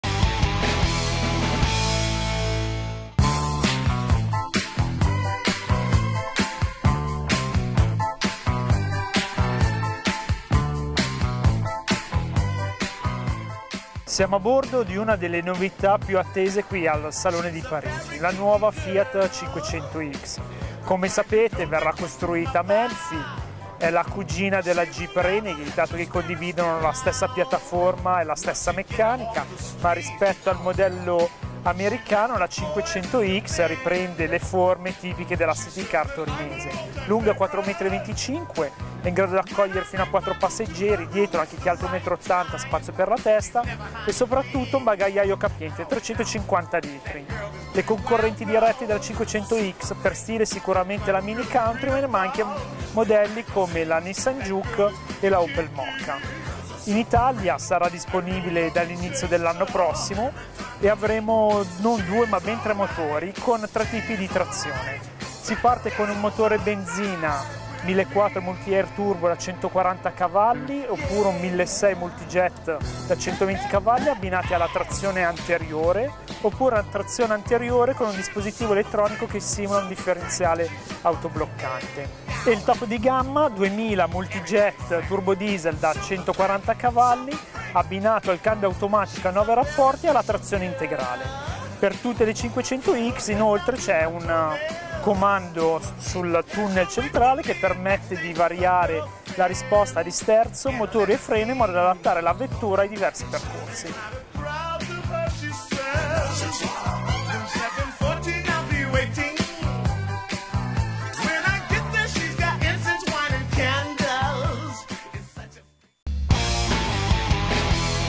FIAT 500X
Ecco la più attesa del Salone: la nuova Fiat 500X